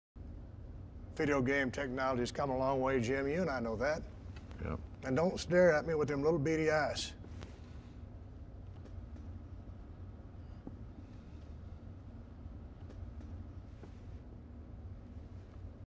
CM Punk and Stone Cold Steve Austin share a cute moment
The real stone cold stunner comes from his smile.